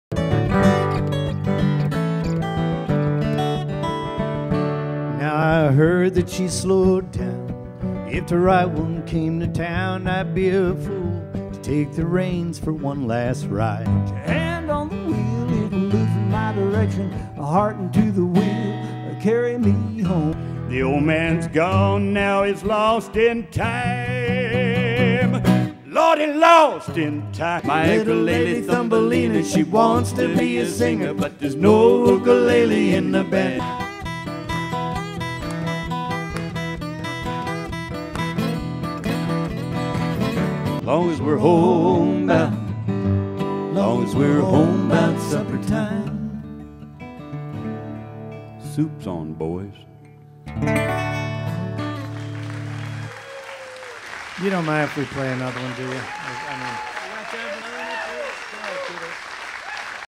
NCTV LIVE Troubadour Concert
TouchDown Productions presents the first LIVE broadcast from the Nevada City Theater.  A Paul Emery Nevada City LIVE event the Troubadours with Peter Wilson, Moe Dixon and Mountain John Hilligoss. Broadcast cast is LIVE on NCTV at 8:00 pm. on Comcast 11, Suddenlink 16 and streamed at Nevada County TV .org.